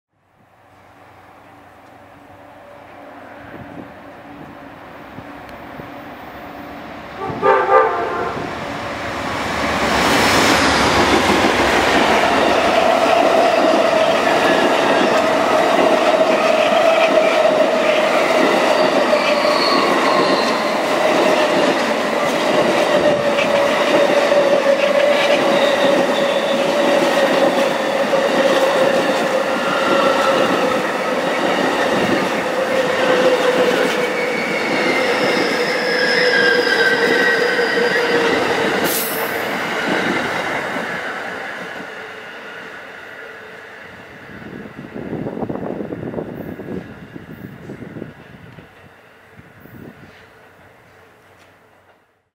Amtrak Xtra #844 w/ Veteran's Unit & New 18 Viewliner II Baggage Cars @ Hastings (Hudson Line) [4k]
Amtrak Extra #844 heads south down Metro North's Hudson Line with 18 brand new Viewliner II Baggage Cars passing Hastings-on-Hudson station on it's way down to Florida. Leading the train are P42's #42 (Verteran;s Unit) & #822 (Phase III Heritage). The train originated at the CAF Plant in Elmira, NY earlier that day and is set to arrive in Florida two days after. Once there the cars will go into the acceptance phase an put on the Silver services for their first revenue runs. (Taken 12/17/14)